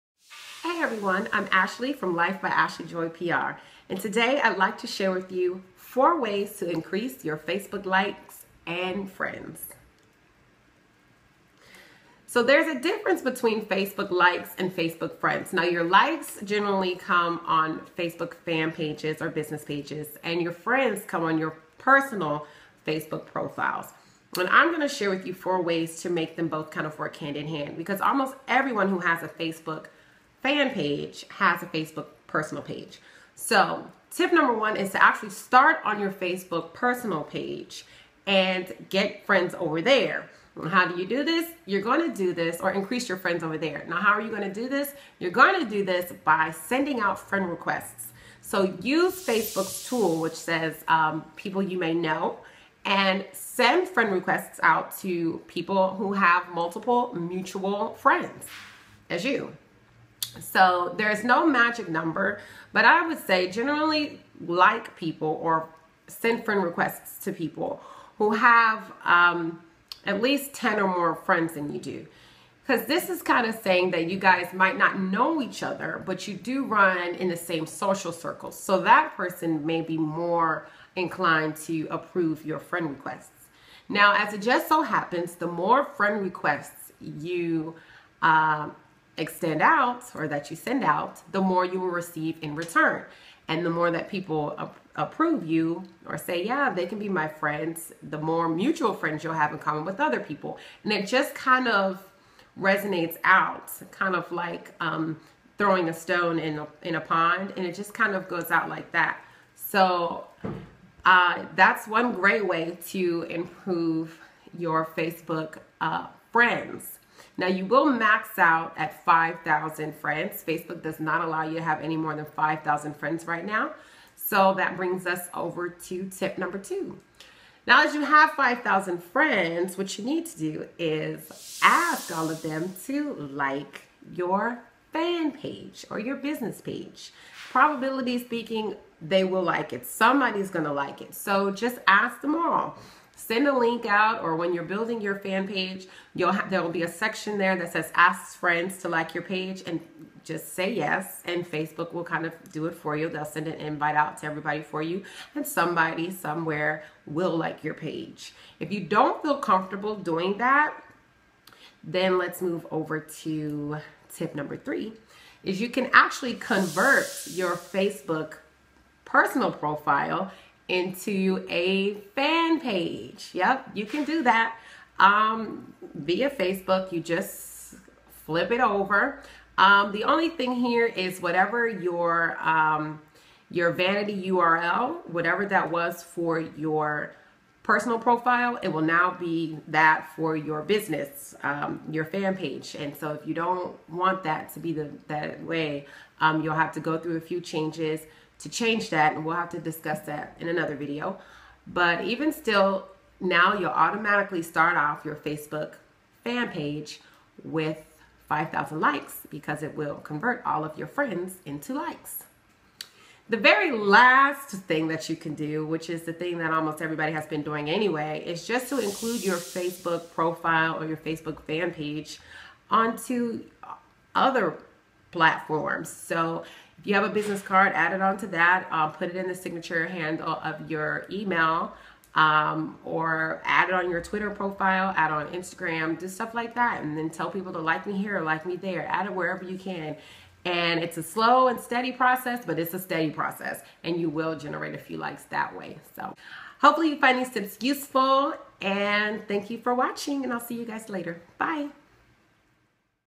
4 Ways To Increase Your Facebook Likes And Friend.
Want to know how to increase your Facebook likes for your fan page or friends on your profile page? Well, watch this video to find out how to do both in only four easy steps!